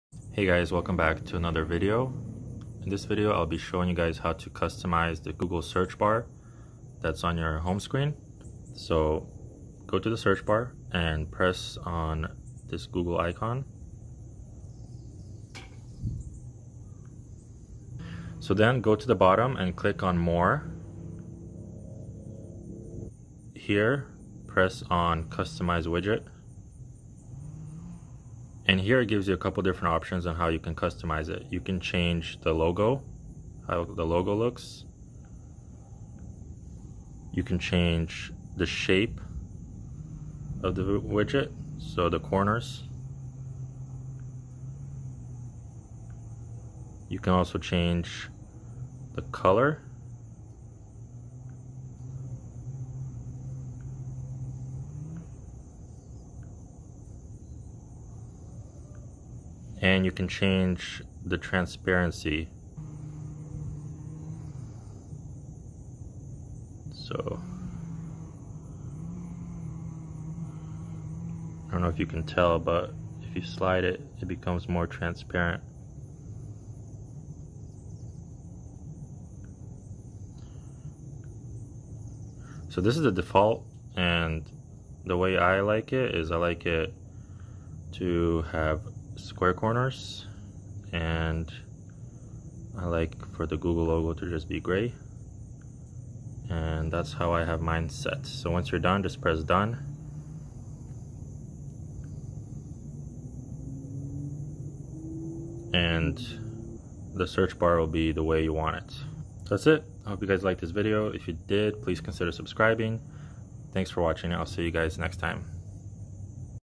Customize Google Search Bar On Samsung Galaxy S8 S9 S10 S20
This video explains how to customize the Google search bar on the home screen of your phone. You can change the color, shape, and logo to fit your personal taste. This was done on the Samsung S8+ with Android 9. I also checked if this works on the S20+ with Android 10 and it does.